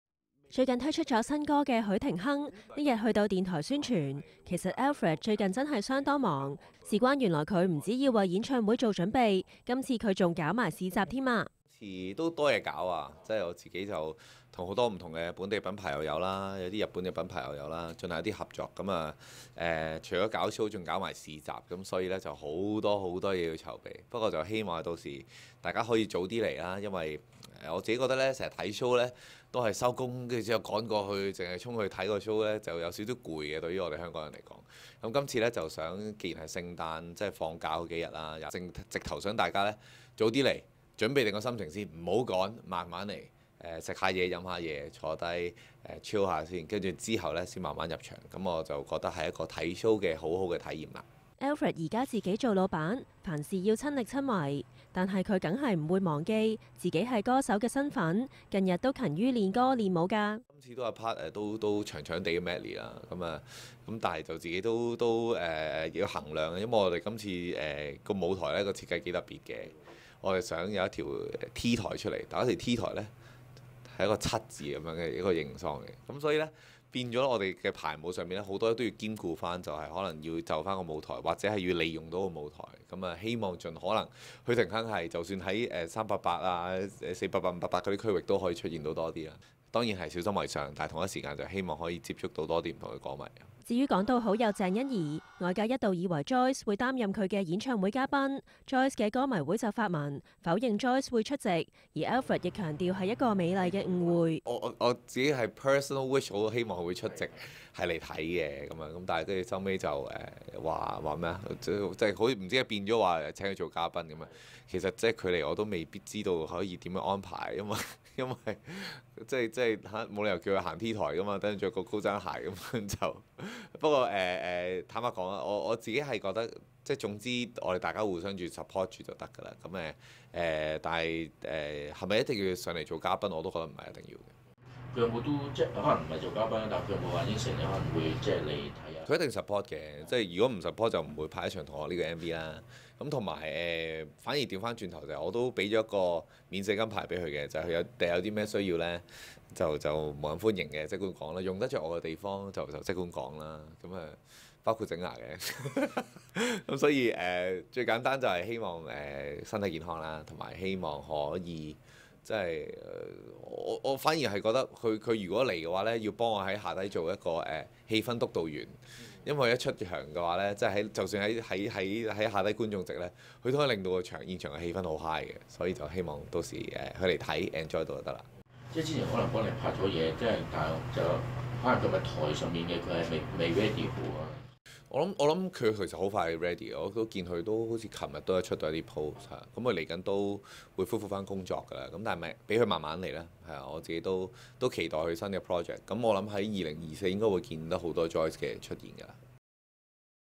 娛樂新聞 ｜ Alfred許廷鏗盼樂迷有不一樣體驗 與Joyce互相支持已足夠 ｜ Alfred ｜ 許廷鏗 ｜ Joyce ｜ 鄭欣宜 ｜ 欣宜 ｜ HOYTV｜HOY77
Alfred許廷鏗於聖誕舉行演唱會，一直盛傳好友Joyce鄭欣宜會擔任嘉賓，搞到Joyce嘅歌迷會發文否認，Alfred強調係一個美麗嘅誤會，不過可以肯定嘅係Joyce一定會來支持，佢希望對方擔任氣氛督導員，有對方嘅地方一定會氣氛高漲啊﹗